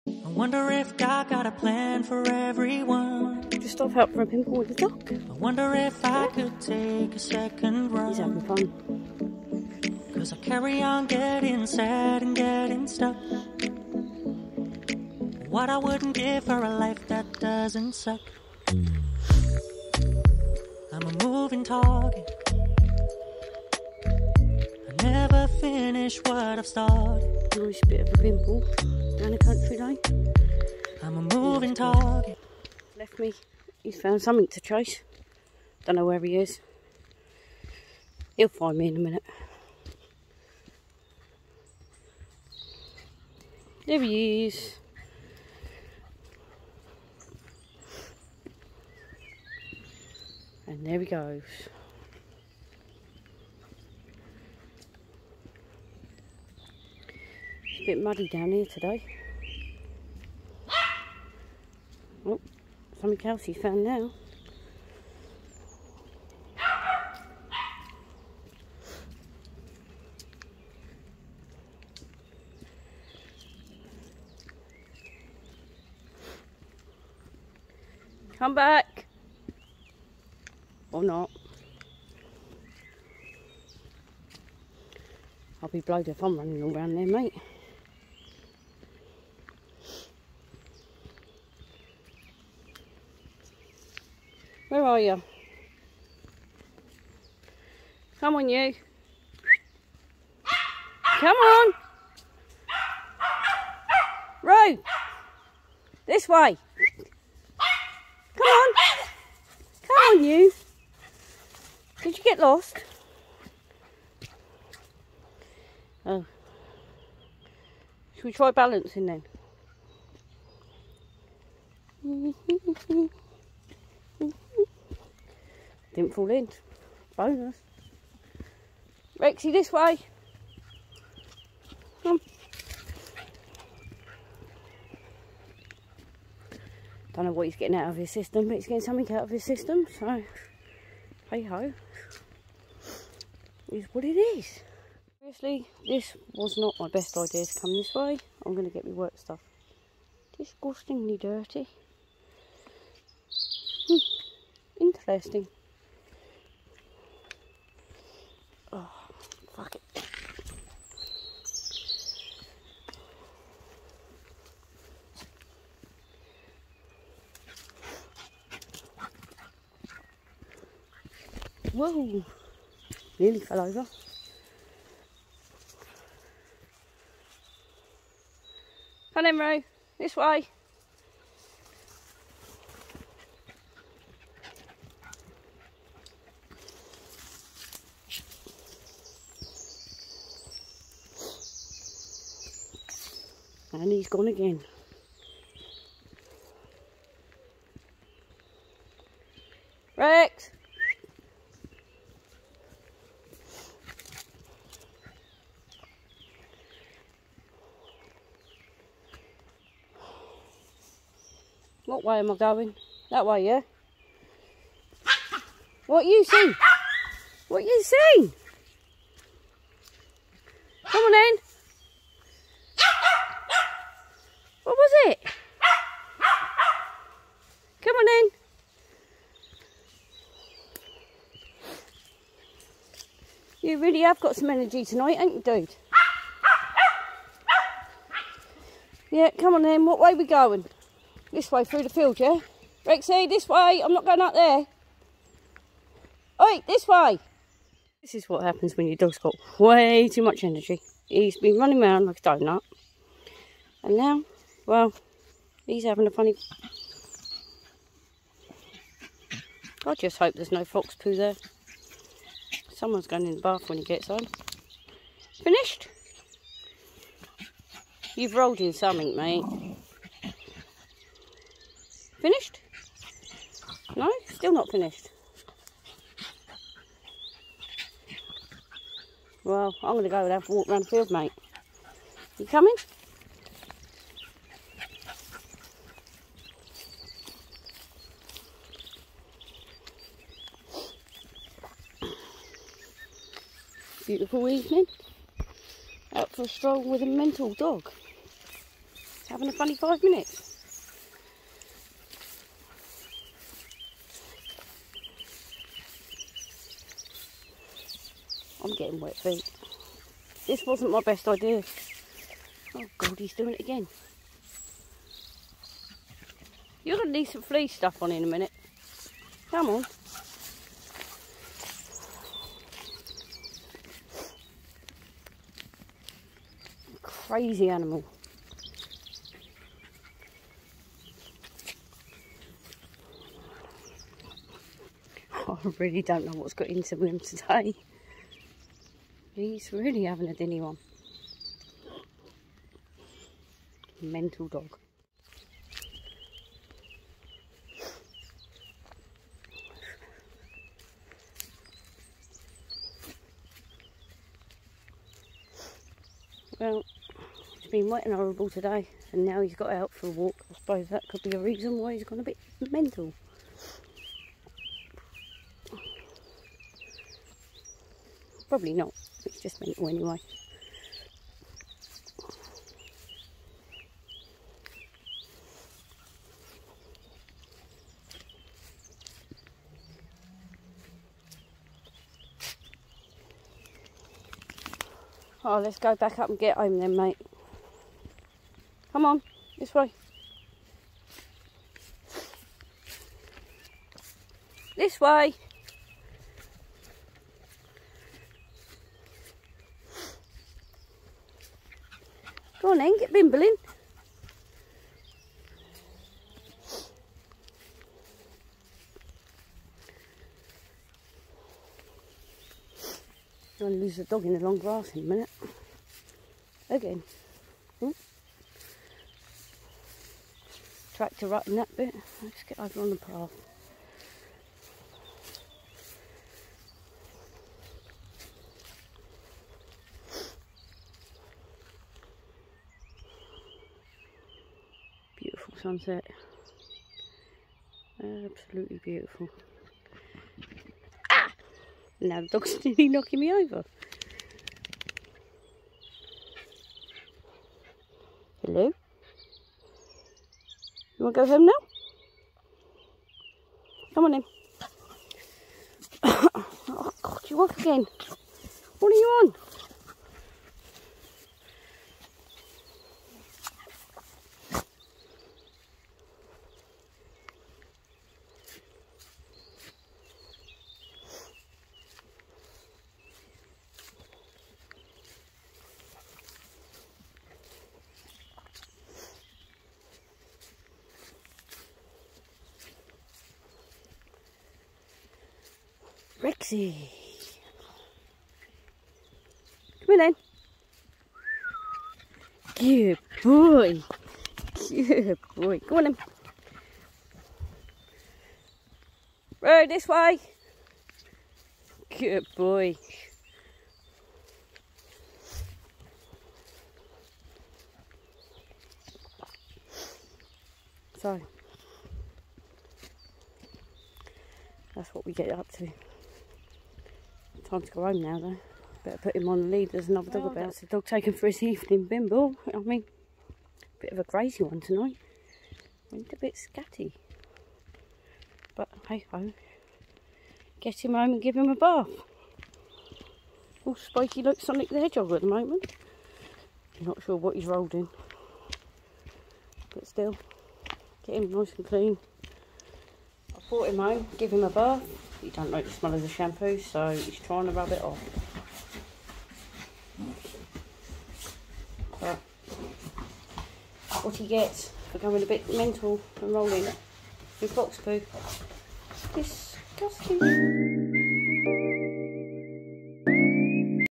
Jack Russell walk and talk
Taking the dog for a country walk, he loved it, needed a bath when he got home, crazy dog